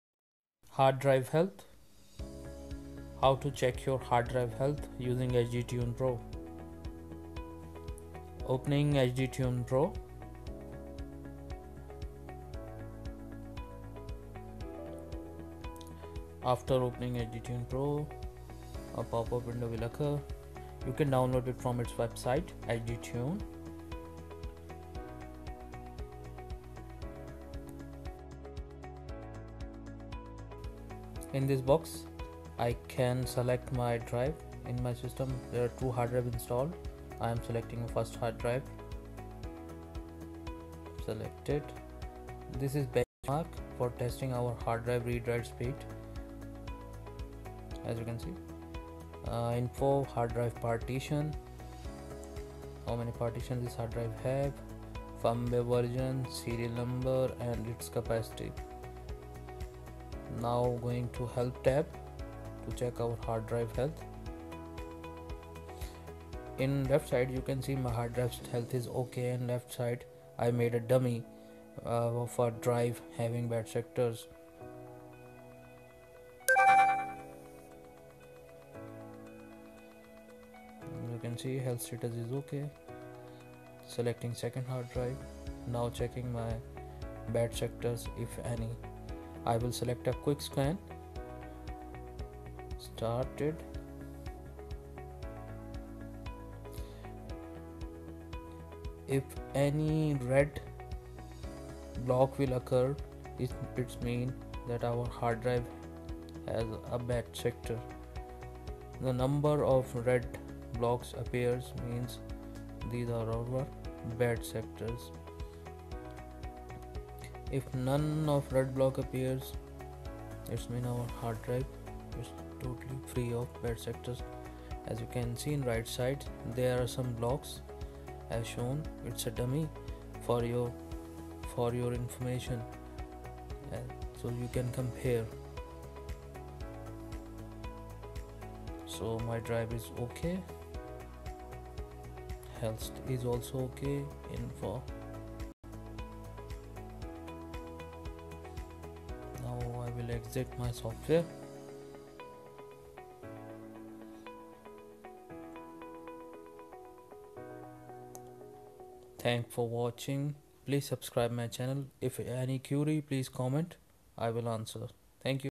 HOW TO CHECK HARD DRIVE HEALTH | HD TUNE
The easy way to check your hard drive hard disk is by using hd tune software it will give you all information about hard drive